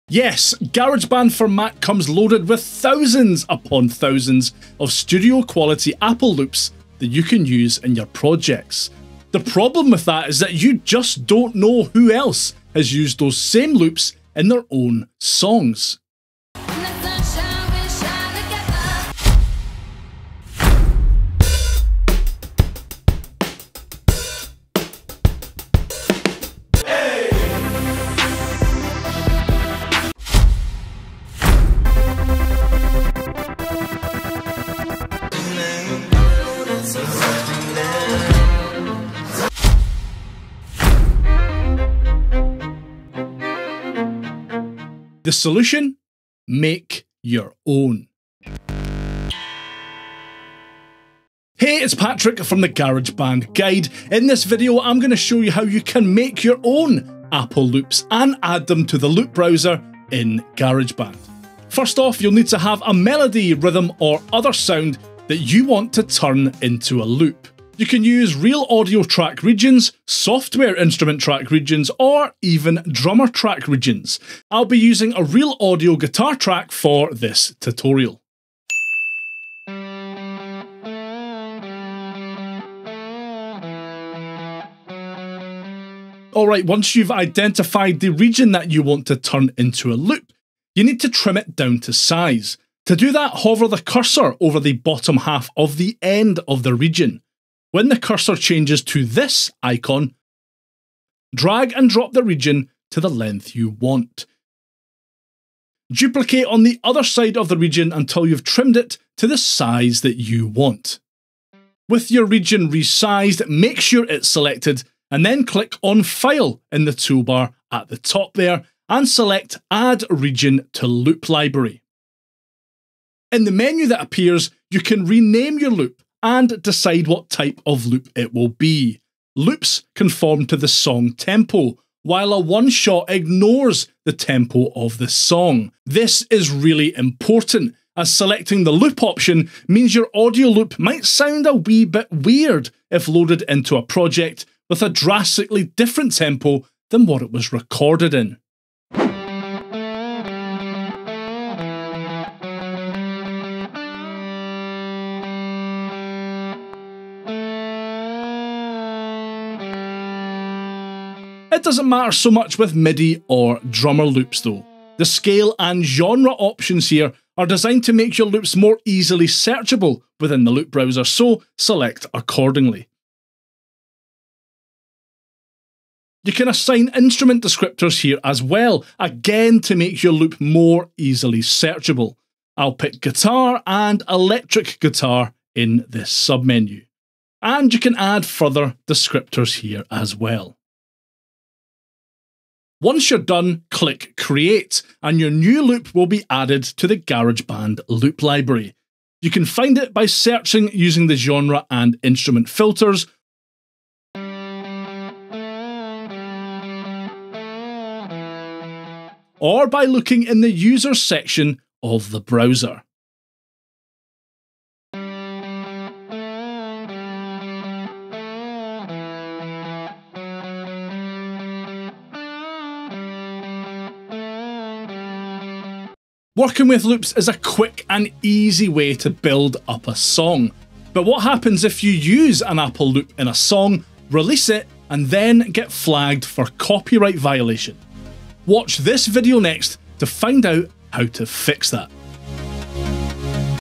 How to make your own Apple Loops in GarageBand
GarageBand Loops suck. Make your own. // GarageBand Loops Tutorial

GarageBand for Mac comes loaded with thousands upon thousands  of studio quality apple loops that you can use in your projects.

The problem with that is that you just don’t know who else has used those same loops in their songs.

The solution? Make your own.

———FREE GUIDES———

———GEAR———

———SOCIALS———